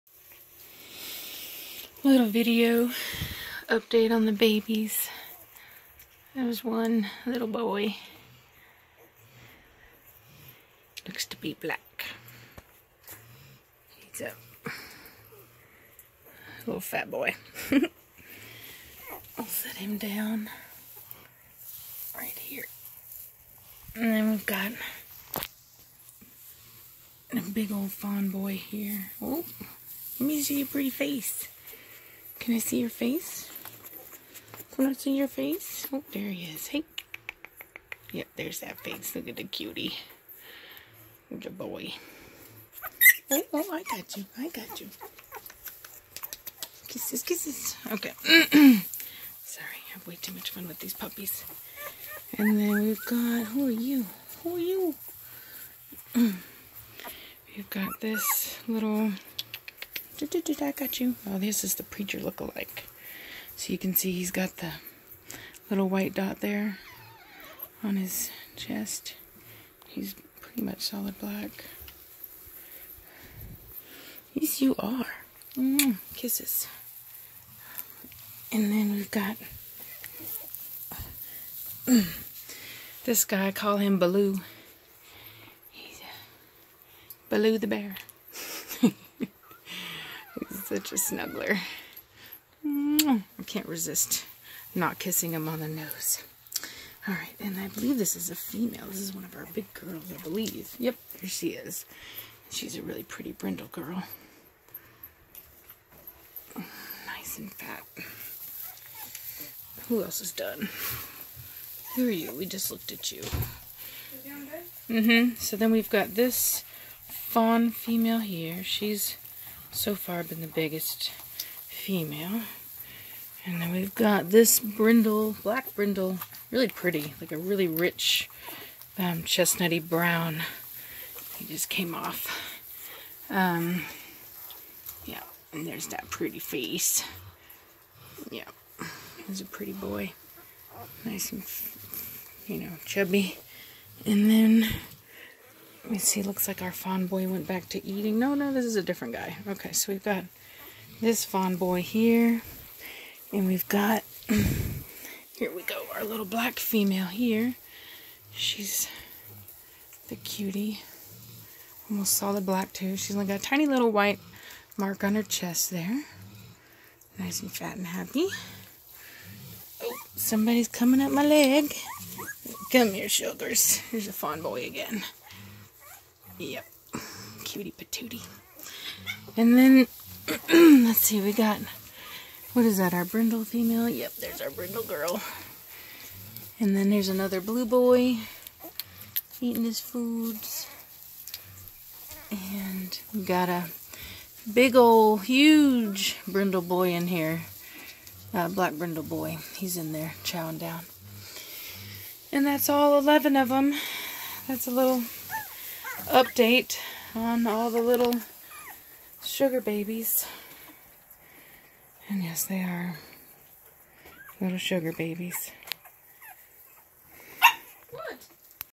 Cane Corso Puppies 1 week old update
This is just a video update of our puppies. They turned a week old yesterday. Follow us on...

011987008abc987